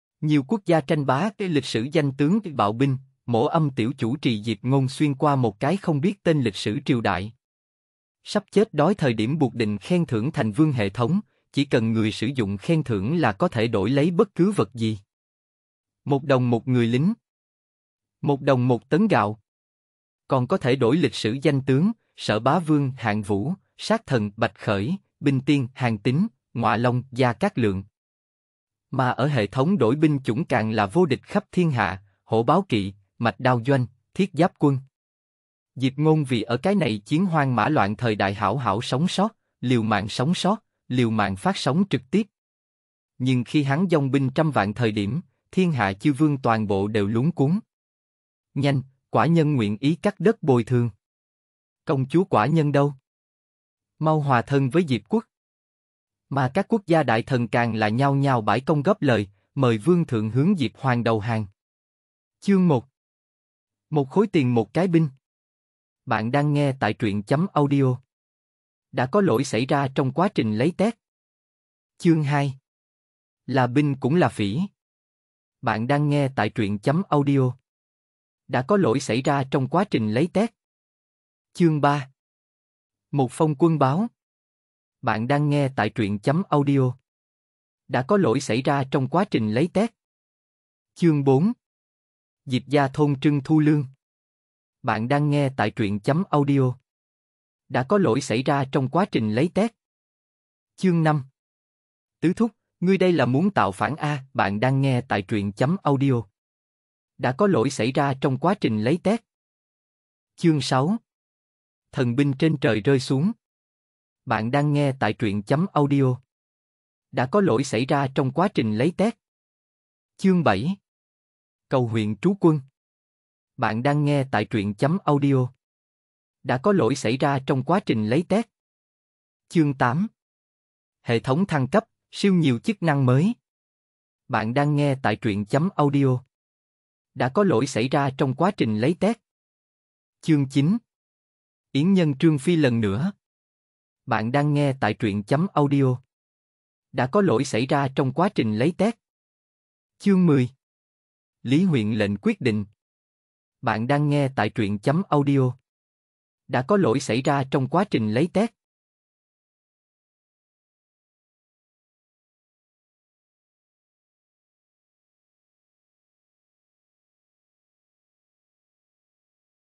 Khen Thưởng Một Nguyên Một Cái Binh, Nữ Đế Cầu Ta Đừng Tạo Phản Chương 1-10 FULL | Lịch Sử Brain Hol
- w - đọc truyện chữ + audio

00:00:00 Chương 1: Một khối tiền một cái binh!
00:01:11 Chương 2: Là binh cũng là phỉ
00:01:21 Chương 3: Một phong quân báo
00:01:30 Chương 4: Diệp Gia thôn trưng thu lương
00:01:40 Chương 5: Tứ thúc, ngươi đây là muốn tạo phản a!
00:01:50 Chương 6: Thần binh trên trời rơi xuống
00:01:59 Chương 7: Cầu huyện trú quân
00:02:09 Chương 8: Hệ thống thăng cấp, siêu nhiều chức năng mới
00:02:19 Chương 9: Yến Nhân Trương Phi lần nữa
00:02:29 Chương 10: Lý Huyện lệnh quyết định

Intro:

(nhiều quốc gia tranh bá+lịch sử danh tướng+bạo binh)

Mỗ âm tiểu chủ trì Diệp Ngôn xuyên qua một cái không biết tên lịch sử triều đại.

Sắp chết đói thời điểm buộc định khen thưởng thành vương hệ thống, chỉ cần người sử dụng khen thưởng là có thể đổi lấy bất cứ vật gì.

Một đồng một người lính! Một đồng một tấn gạo!

Còn có thể đổi lịch sử danh tướng, "Sở Bá Vương" Hạng Vũ, "Sát Thần" Bạch Khởi, "Binh Tiên" Hàn Tín, "Ngọa Long" Gia Cát Lượng...

Mà ở hệ thống đổi binh chủng càng là vô địch khắp thiên hạ, Hổ Báo Kỵ, Mạch Đao Doanh, Thiết Giáp Quân...

Diệp Ngôn vì ở cái này chiến hoang mã loạn thời đại hảo hảo sống sót, liều mạng sống sót, liều mạng phát sóng trực tiếp.

Nhưng khi hắn dong binh trăm vạn thời điểm, thiên hạ chư vương toàn bộ đều luống cuống.

Nhanh, quả nhân nguyện ý cắt đất bồi thường!

Công chúa quả nhân đâu! Mau hòa thân với Diệp quốc.

Mà các quốc gia đại thần càng là nhao nhao bãi công góp lời, "Mời vương thượng hướng Diệp Hoàng đầu hàng!"

Bạn đang nghe Khen Thưởng Một Nguyên Một Cái Binh, Nữ Đế Cầu Ta Đừng Tạo Phản Tập 1 chương 1 đến 10